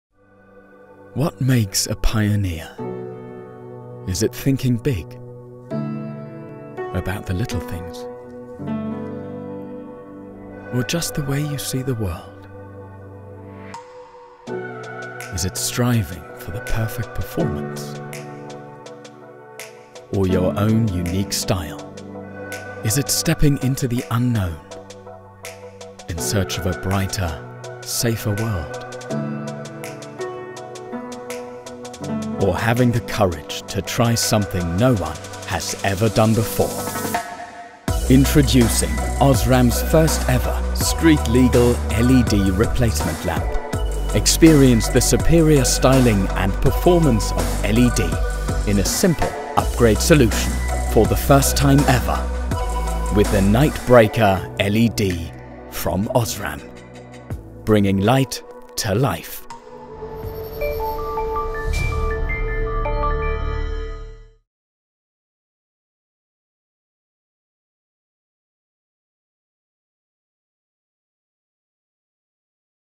Pioneering innovation that turns heads – The 1st NIGHT BREAKER LED!
An eye-catching revolution in technology and design: OSRAM's first street-legal LED replacement lamp , the NIGHT BREAKER® H7-LED*. The superior performance of the most advanced lighting technology offers up to 220% brighter light** and higher contrast. Now you can see further and react faster. These vibration-resistant and energy-efficient LED replacement lamps are designed to last up to 5 times longer***. Take the design of your vehicle to the next level with these attractive LED replacement lamps.

*Only applies to the respective countries in which there is approval and the vehicle models and light functions currently listed in the compatibility list.
** Compared to minimum ECE R112. On top with 50% less glare.
*** Compared to halogen lamps.

Your car deserves OSRAM – the No. 1 in Automotive Lighting!